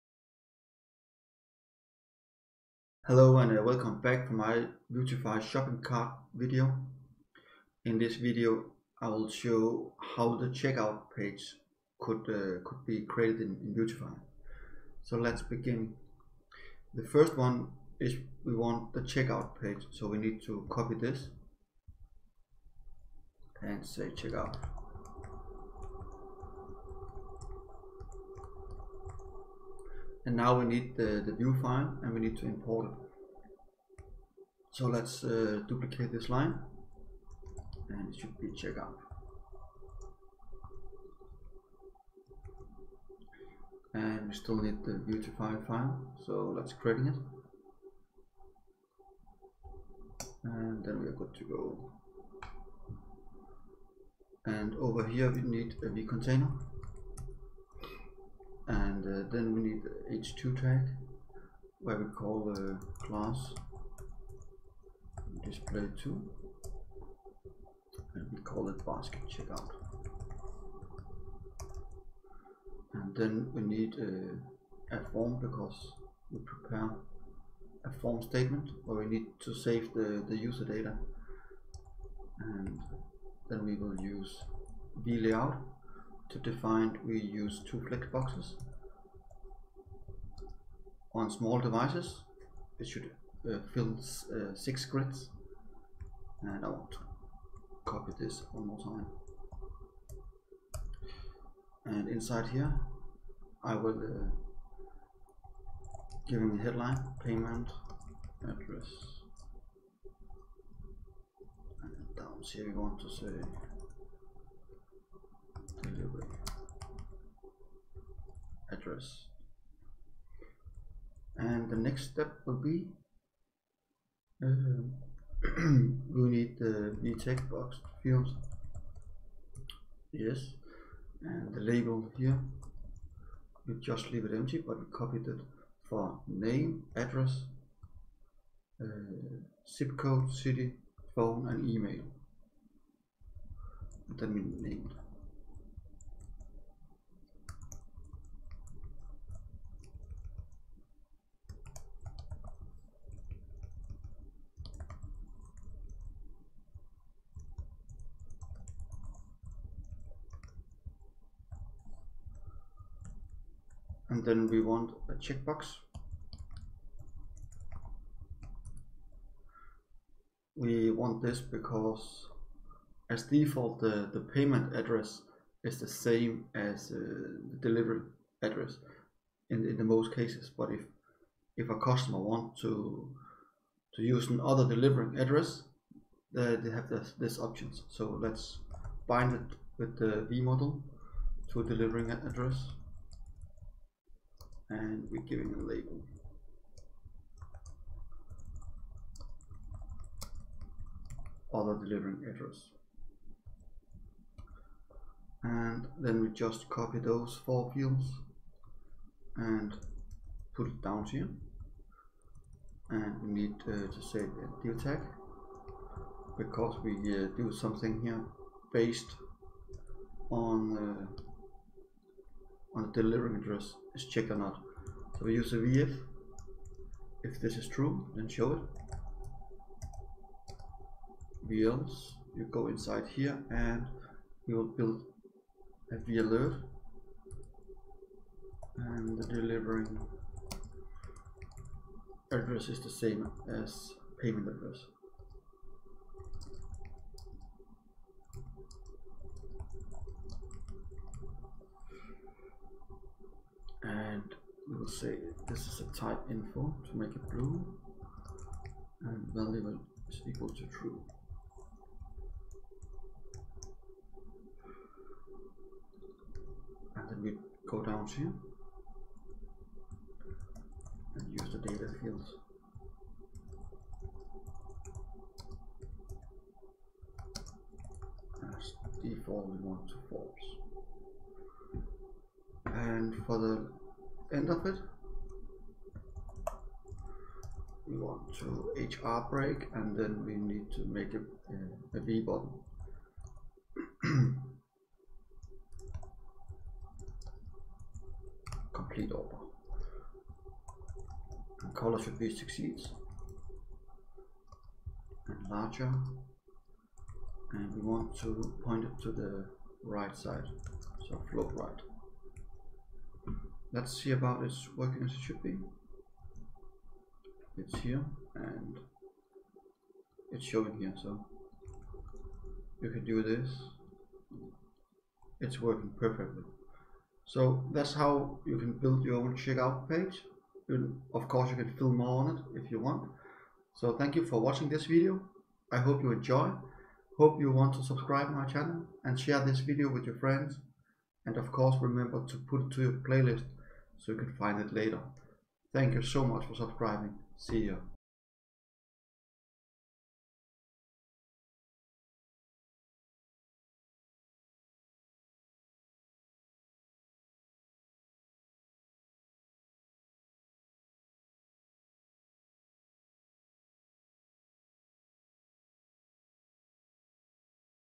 Vuetify shopping cart - Create shopping basket checkout page
I will show you in this vuetify shopping cart tutorial how i create a basket checkout page and how you can use vueitfy ui when you code with vue-cli.

i will using v-container with v-layout and v-flex where i build with v-text fields, see my vide to understand how its working.